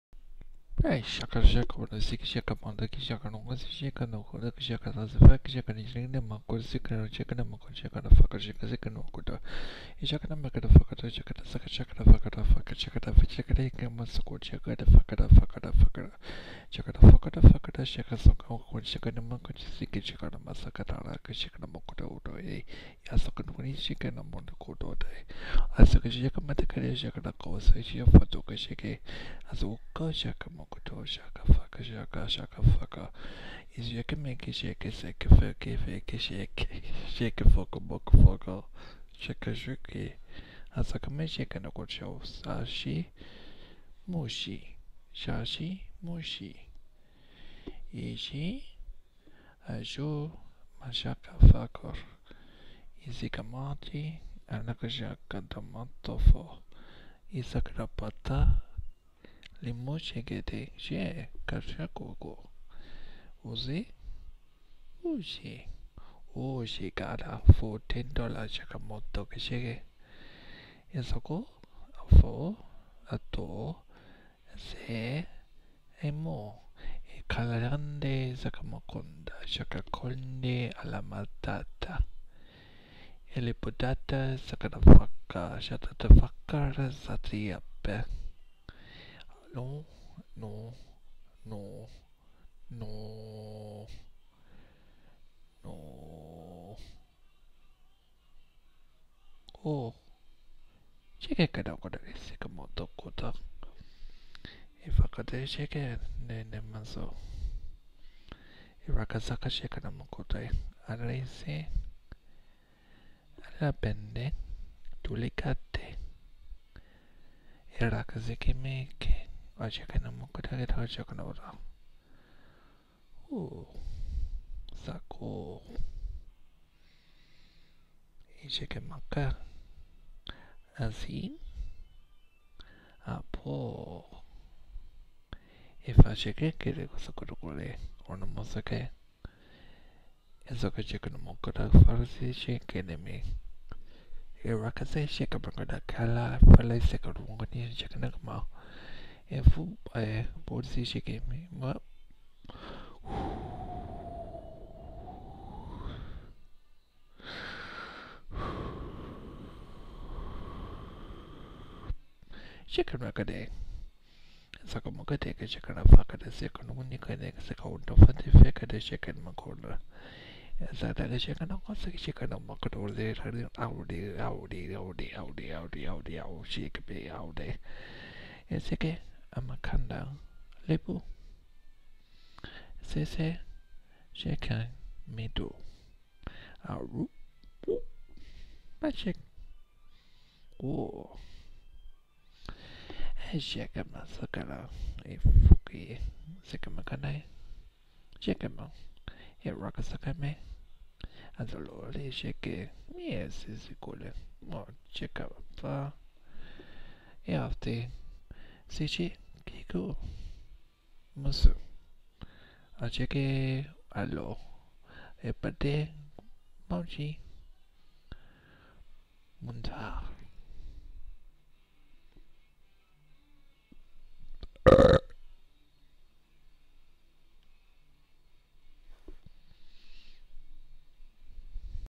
SubGenius Séance
I had no idea this happened to me until someone sent me the video. I do not know if I was summoning J.R. "Bob" Dobbs, an Xist, or if I was having a psychotic episode, either way this video MUST BE SHARED WITH THE VIEWING PUBLIC.